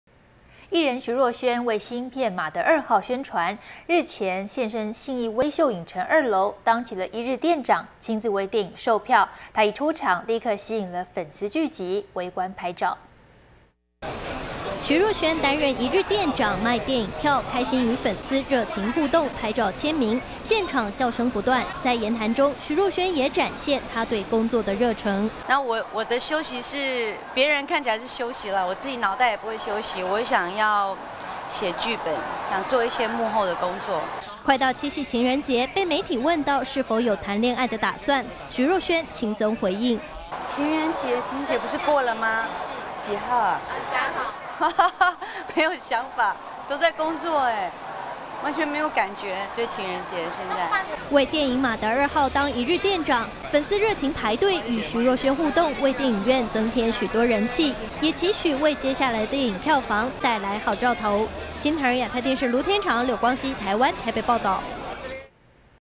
【徐若瑄_娛樂新聞】徐若瑄為電影宣傳 體驗一日店長
【徐若瑄_娛樂新聞】徐若瑄為電影宣傳 體驗一日店長(NTDAPTV) 藝人徐若瑄為新片〈瑪德2號〉宣傳，今天下午現身信義威秀影城2樓，當起一日店長，親自為電影售票。一出場立刻吸引粉絲聚集，圍觀拍照。【新唐人亞太台2013年8月11日訊】

徐若瑄擔任一日店長賣電影票，開心與粉絲熱情互動、拍照簽名，現場笑聲不斷。在言談中，徐若瑄也展現她對工作的熱誠。

徐若瑄: 「我的休息是別人看起來是休息我腦袋也不會休息我想要寫劇本做一些幕後的工作恩還沒想好。」

快到七夕情人節，被媒體問到是否有談戀愛的打算，徐若瑄輕鬆回應。

徐若瑄:「情人節不是過了嗎?幾號啊?沒有想法都在工作，完全沒有感覺，對情人節現在。」

為電影瑪德二號當一日店長，粉絲熱情排隊與徐若瑄互動，為電影院添許多人氣，也期許接下來的電影票房有好兆頭。